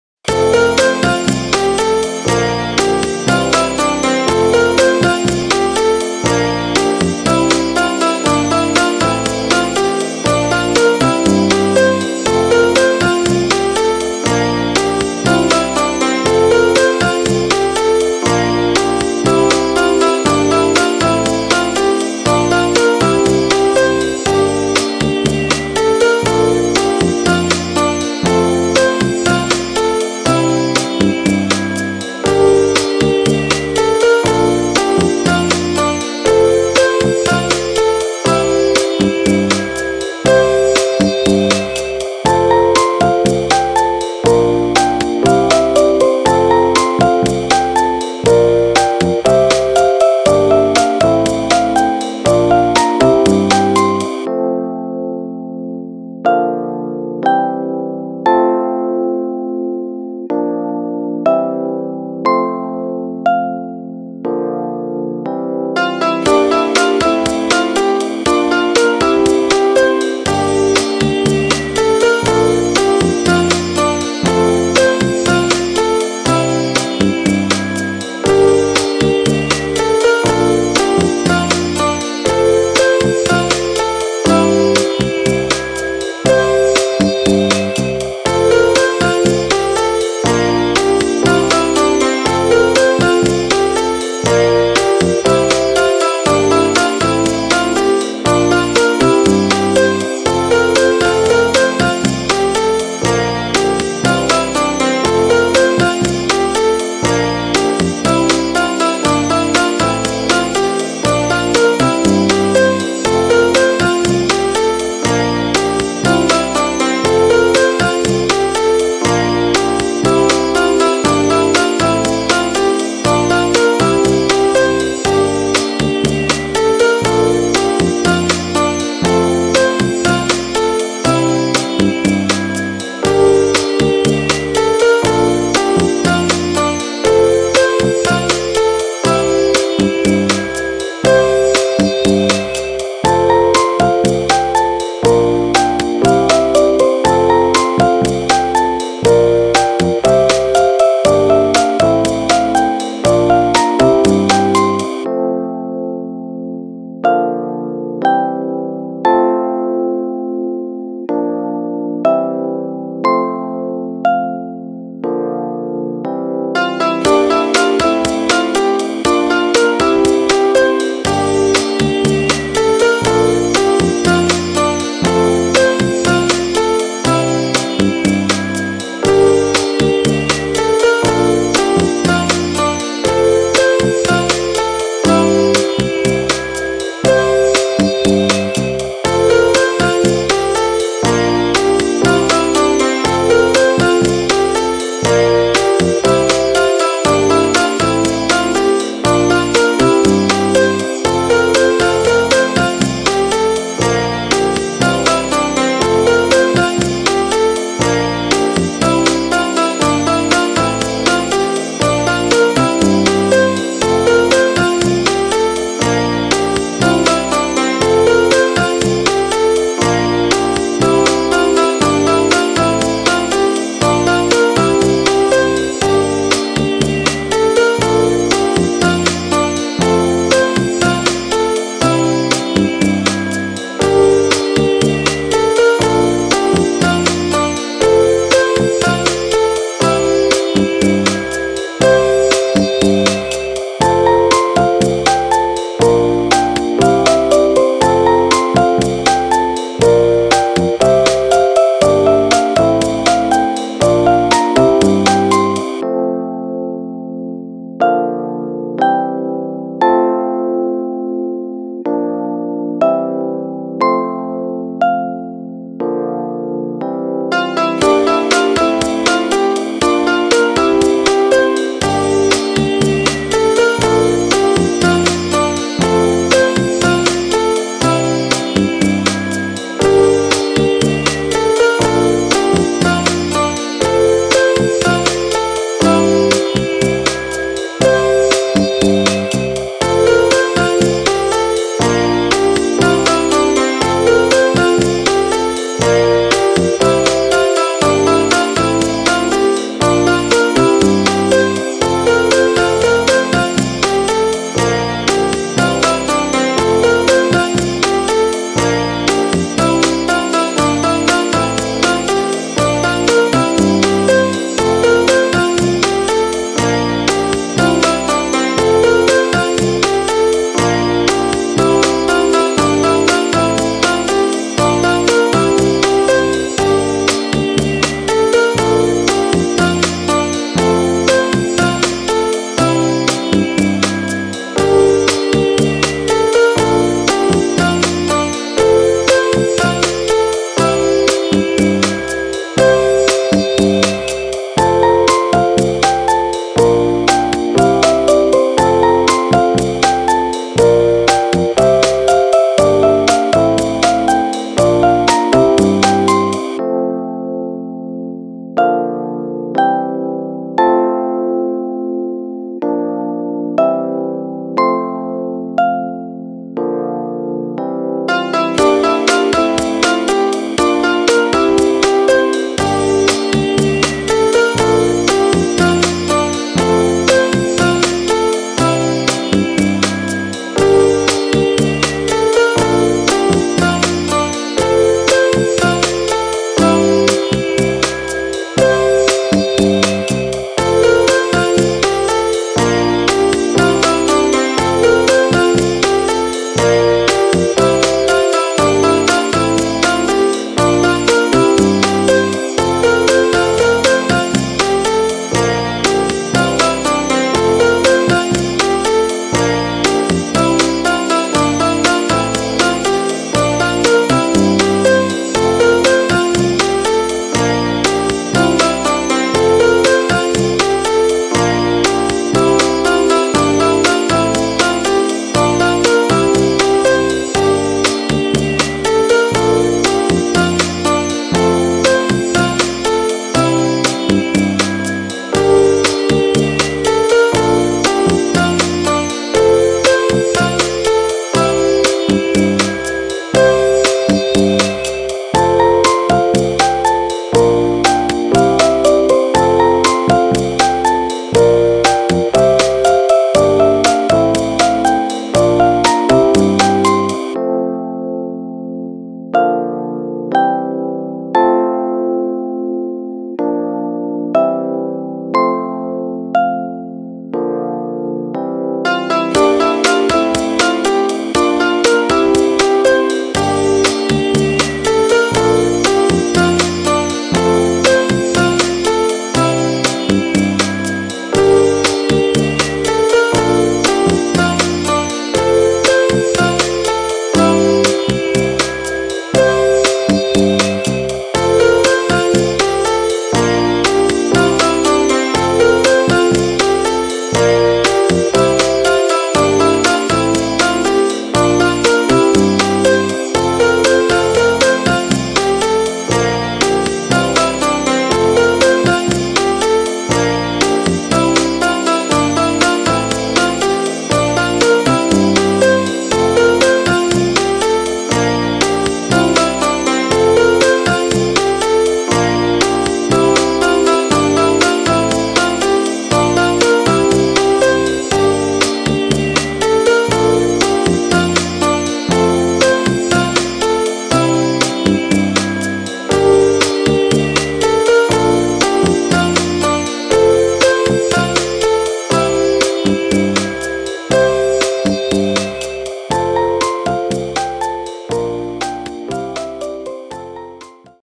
Drawing for kids Princess Jasmine
Independent and proud daughter of the Sultan, Jasmine became the first princess in the universe of "Disney", which is endowed with oriental appearance.

Drawing princess Jasmine.